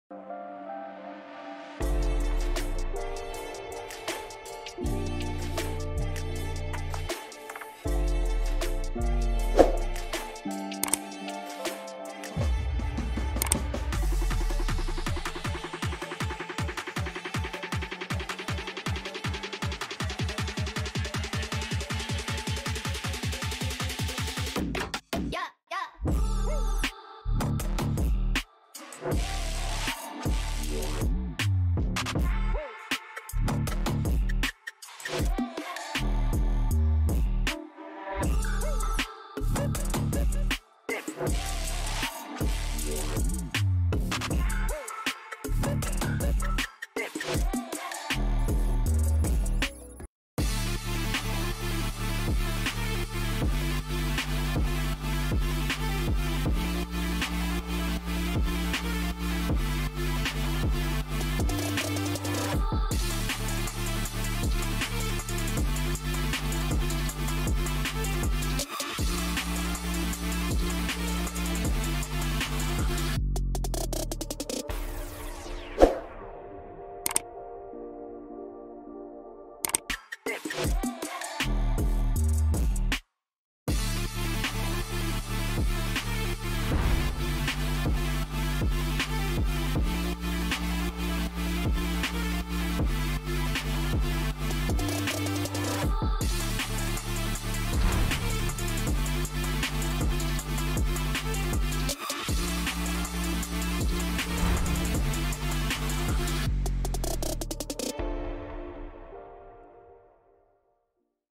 3D Printed Roof Box Design // 1:10 scale RC Defender accessories
A custom Classic D110 with a custom 3D printed Roof box.